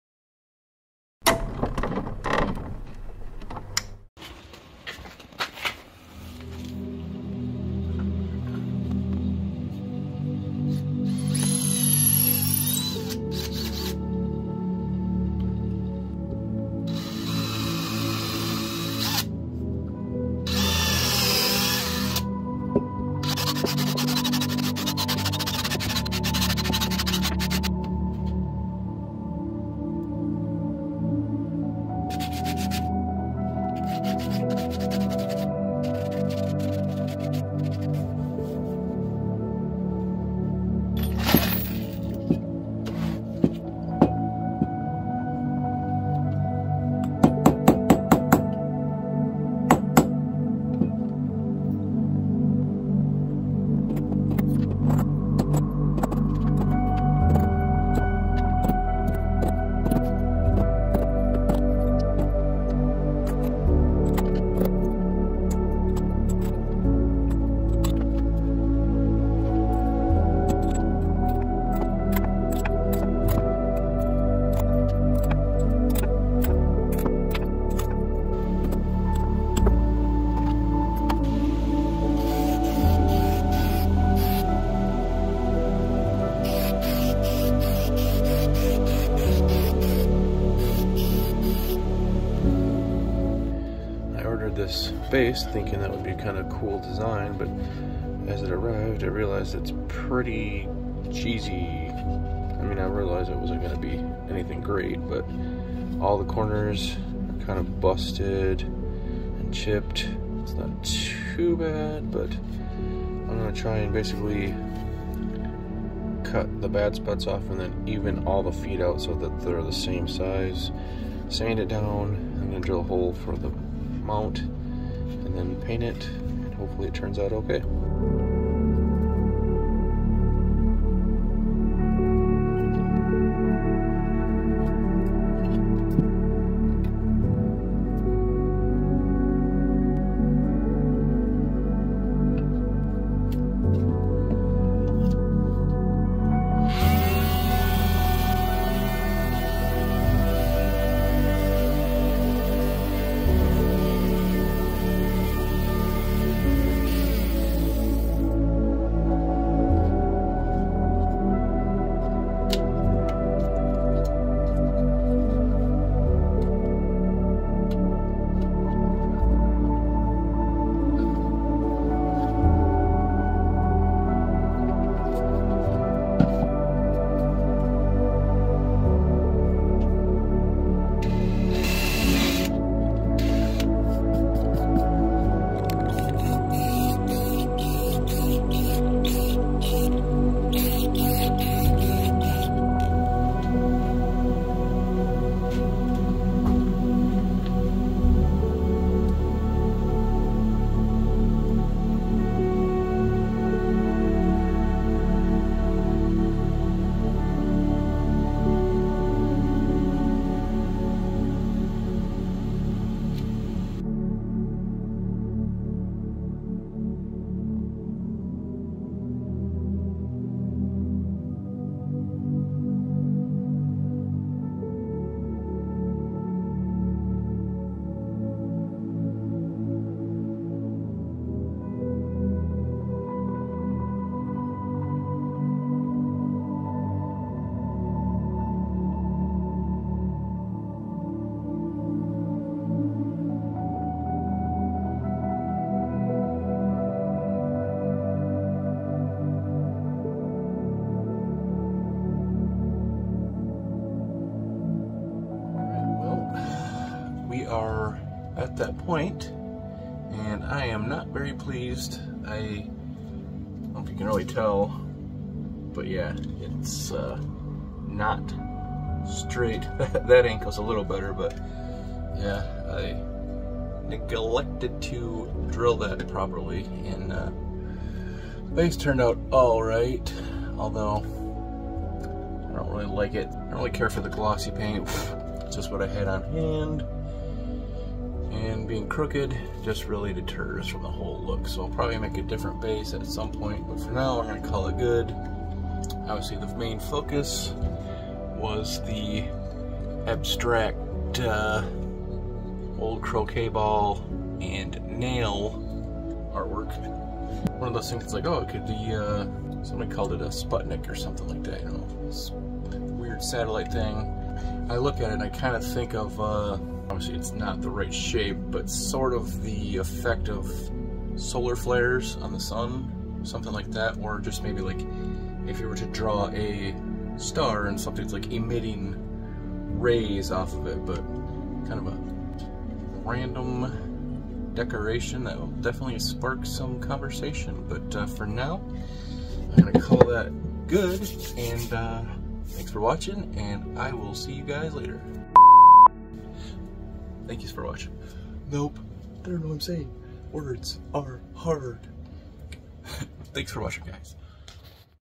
Salvage to Sculpture
An old wooden croquet ball, and old rusty nails repurposed into art.

wood base